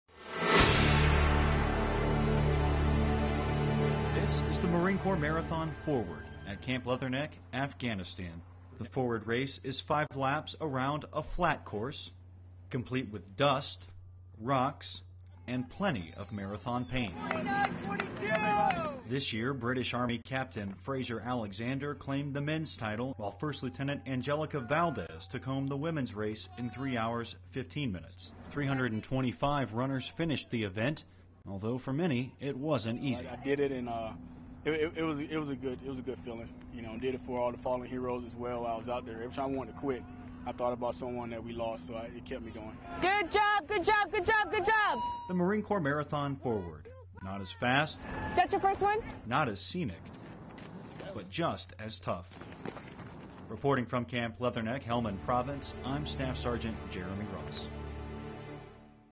Deployed Marines run Marine Corps Marathon
More than 30,000 people ran this year's Marine Corps Marathon in the nation's capital. Staff Sgt. Jeremy Ross tells us how the race wasn't limited to stateside runners.